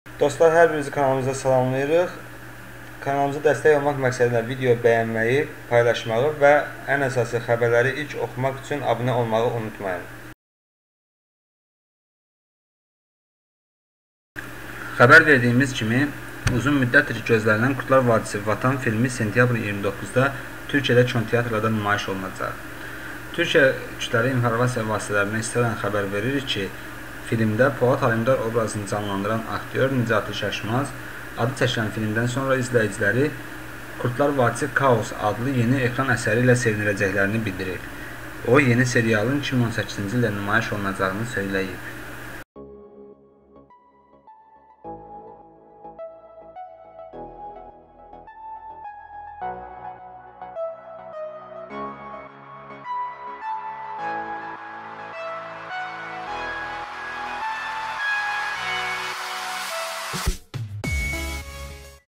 “Kurtlar Vadisi Kaos” başlayır
► Xəbər verdiyimiz kimi, uzun müddətdir gözlənilən “Kurtlar Vadisi Vatan” filmi sentyabrın 29-da Türkiyədə kinoteatrlarda nümayiş olunacaq.

Oxu.Az Türkiyə KİV-nə istinadla xəbər verir ki, filmdə Polat Alemdar obrazını canlandıran aktyor Necati Şaşmaz adıçəkilən filmdən sonra izləyiciləri “Kurtlar Vadisi Kaos” adlı yeni ekran əsəri ilə sevindirəcəklərini bildirib.

O, yeni serialın 2018-ci ildə nümayiş olunacağını söyləyib.

► Bakıda cinsi azlıq nümayəndələri iş adamının başına oyun açdılar

► 3 dostu ilə əlbir olub sevgilisini zorladılar! (zəhmət olmasa abunə ol və paylaş)